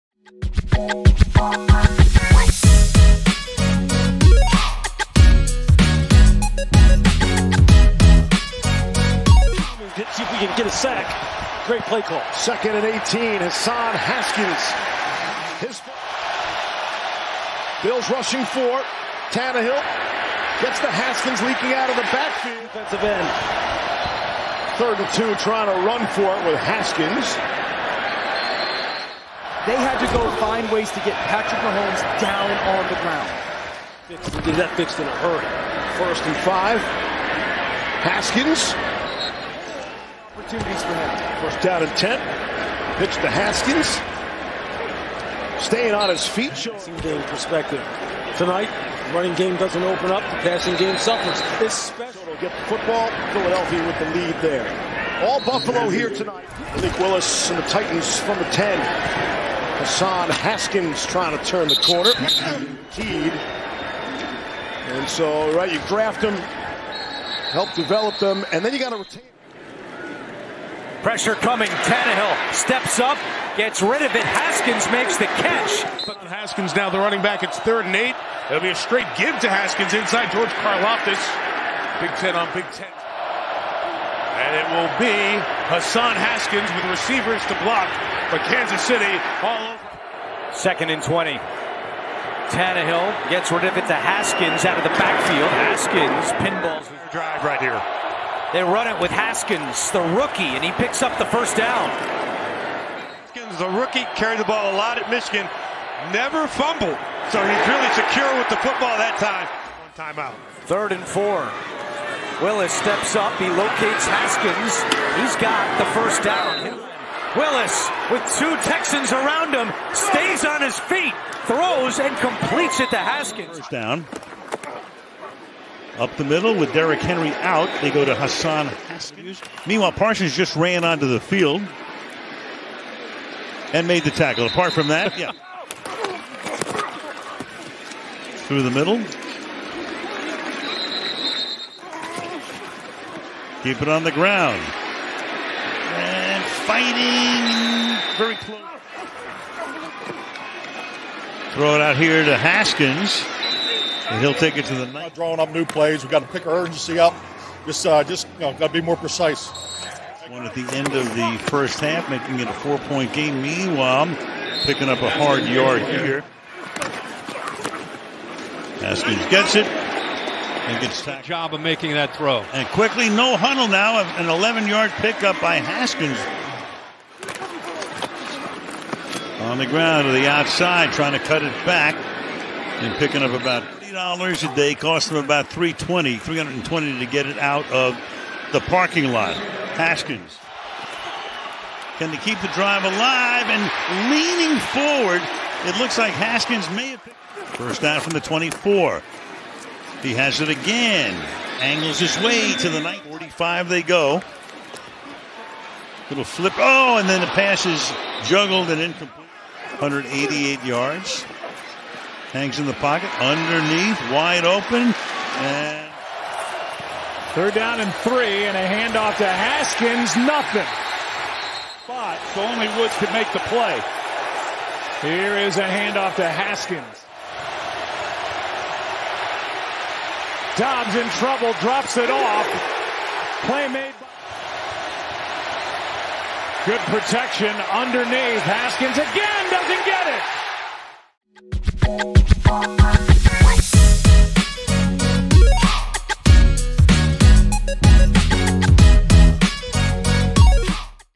Hassan Haskins Full Rookie Season Highlights | Every Run and Target in 2022 | Fantasy Football Film
Hassan Haskins RB Tennessee Titans

2022 Full Rookie Season

Receiving: 12 targets, 11 receptions, 57 yards

00:00 Intro
00:10 Week 2 @ Buffalo Bills
01:09 Week 7 vs Indianapolis Colts
01:16 Week 9 @ Kansas City Chiefs
01:30 Week 15 @ Los Angeles Chargers
01:50 Week 16 vs Houston Texans
02:04 Week 17 vs Dallas Cowboys
03:36 Week 18 @ Jacksonville Jaguars